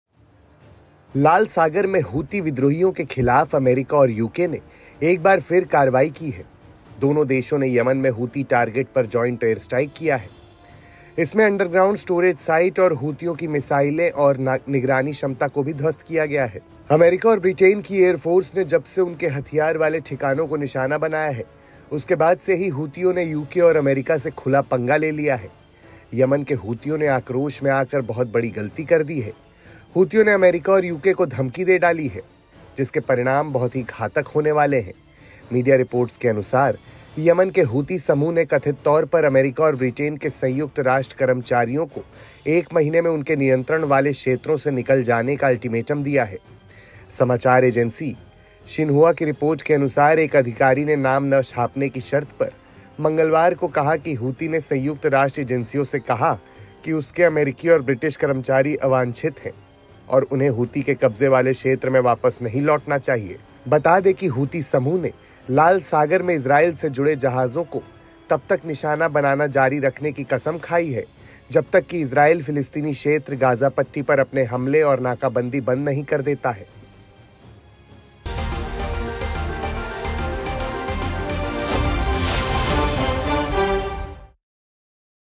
AAJTAK 2 | YEMEN के हूतियों ने छेड़ी खुली जंग, UK - AMERICA को दे दाली धमकी ! | AT2
लाल सागर में हूती विद्रोहियों की ओर से लगातार जहाजों को निशाना बनाया जा रहा है। इससे नाराज अमेरिका और यूके ने एक बार फिर एयर स्ट्राइक की है।अमेरिका और ब्रिटेन की एयरफोर्स ने इस बार उनके हथियार वाले ठिकानों को निशाना बनाया।जिसके बाद अब हूतियों ने यूके और अमेरिका से खुला पंगा ले लिया है।यमन के हूतियों ने आक्रोश में आकर बहुत बड़ी गलती कर दी है।हूतियों ने अमेरिका और यूके को धमकी दे डाली है।जिसके परिणाम बहुत ही घातक होने वाले हैं।

Ships are continuously being targeted by Houthi rebels in the Red Sea. Angered by this, America and the UK have once again carried out an air strike. This time the Air Force of America and Britain targeted their weapon bases. After which now the Houthis have openly messed with the UK and America. The Houthis of Yemen are angry. They have made a big mistake by coming to America. The Houthis have threatened America and the UK. The consequences of which are going to be very fatal.आजतक के साथ देखिये देश-विदेश की सभी महत्वपूर्ण और बड़ी खबरें | Watch the latest Hindi